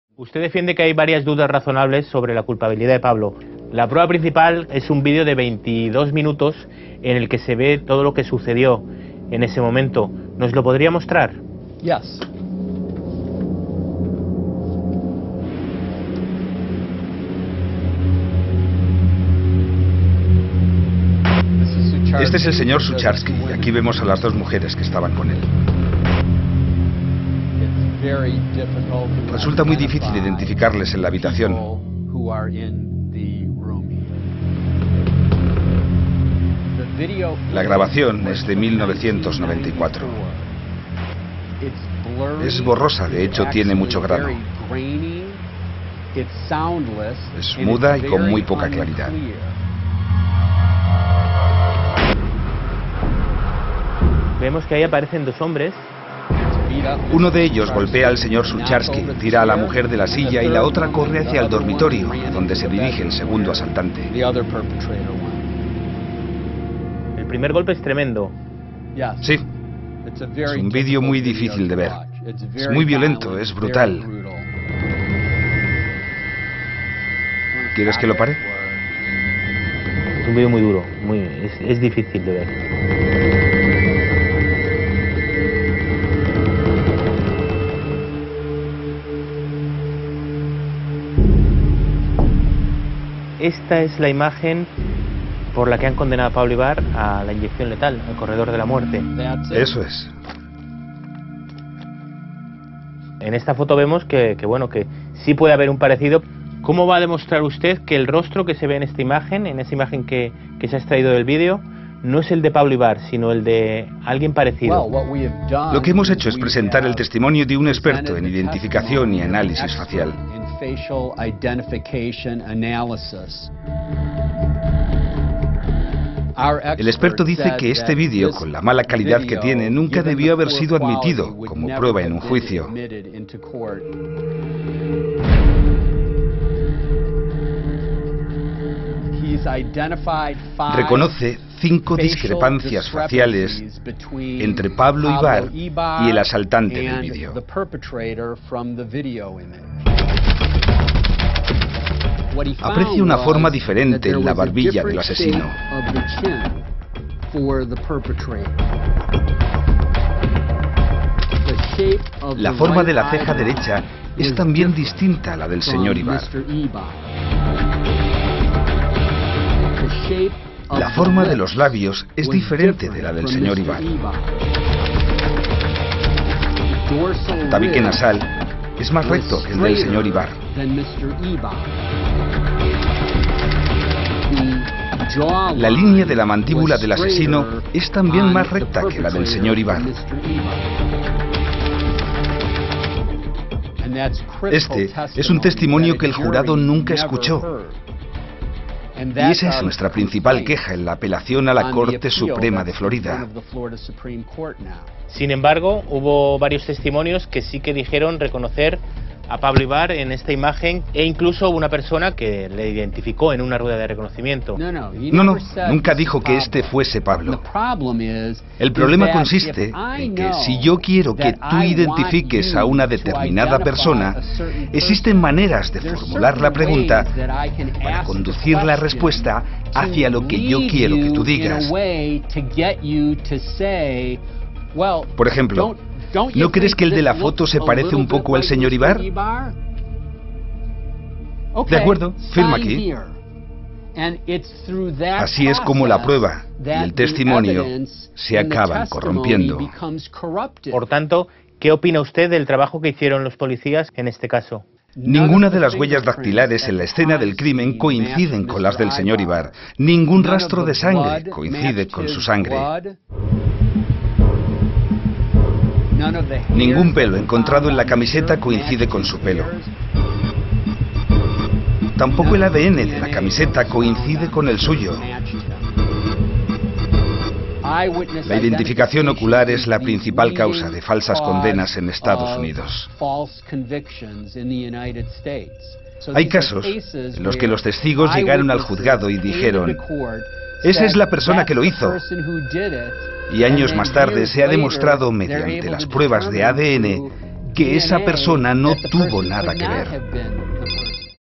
El vídeo que llevó al corredor de la muerte a Pablo Ibar, declarado culpable en EEUU
El jurado ha declarado culpable a Pablo Ibar de tres asesinatos cometidos en Florida en 1994. Una de las pruebas que llevaron al español al corredor de la muerte fue una grabación de 1994 sin audio de 22 minutos.